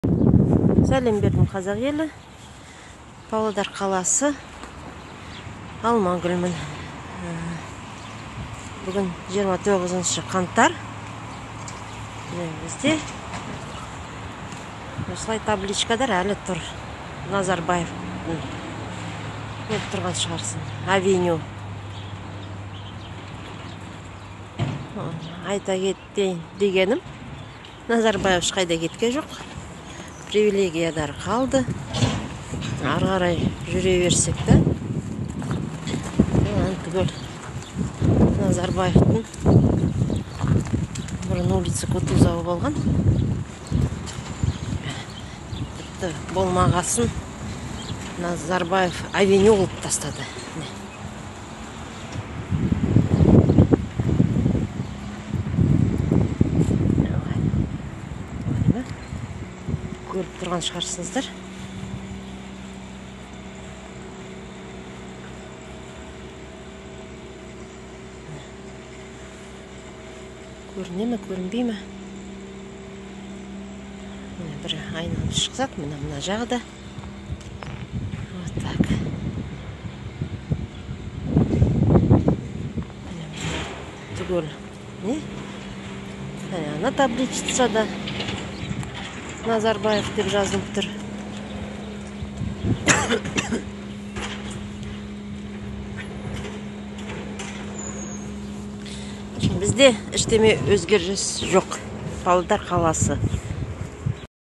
НАЗАРБАЕВ АВЕНЮ В ПАВЛОДАРЕ
Что там Токаев говорил о привилегиях Назарбаева? Типа все, теперь страной рулит он, Токаев? Ну-ну…